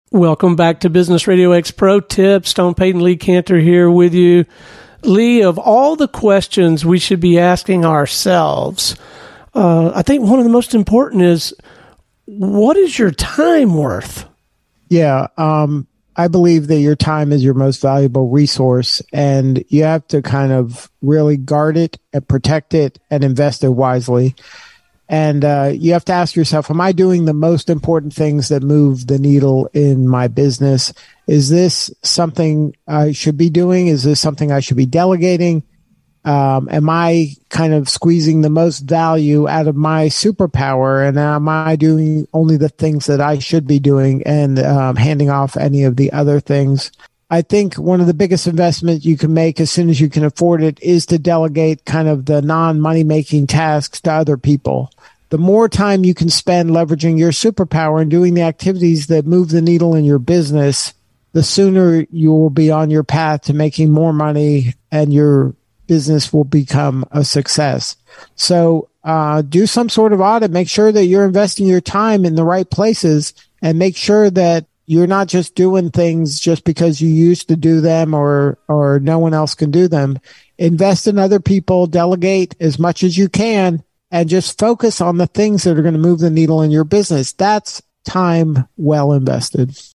BRX Pro Tip: What is Your Time Worth? | Business RadioX® Network
BRX Pro Tip: What is Your Time Worth? Stone Payton: Welcome back to Business RadioX Pro Tips. Stone Payton and Lee Kantor here with you. Lee, of all the questions we should be asking ourselves, I think one of the most important is, what is your time worth? Lee Kantor: Yeah. I believe that your […]
The post BRX Pro Tip: What is Your Time Worth? appeared first on Business RadioX ®.